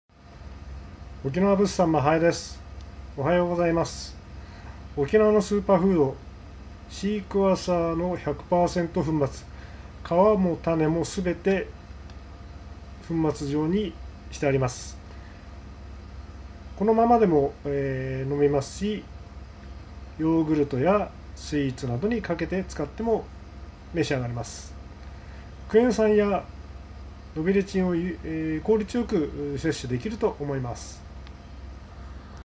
2021年8月30朝、 シークヮーサーパウダー。（ 埼玉県川越市 大正浪漫夢通り 沖縄物産 真南風 （まはえ）より）
皮も種も丸ごと粉末に。
効率よくクエン酸やノビレチンを摂れます。

100%原液より全く酸っぱくありません。

ヨーグルトやスイーツにかけたり、そのまま健康食品として飲んでも。